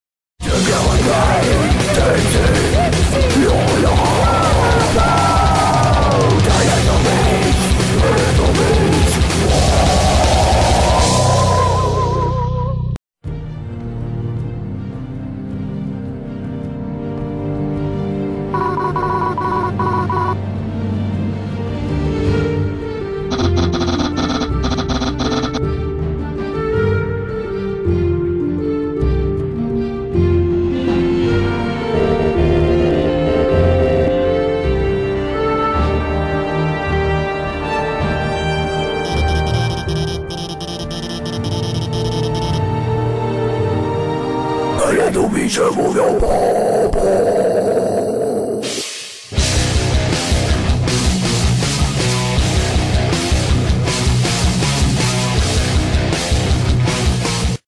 Swapfell in Taiga
One day in the world of Swapfell there was a spruce which had symbolism from Nirn. And then the characters from Swapfell examined it and found out that there is a different world.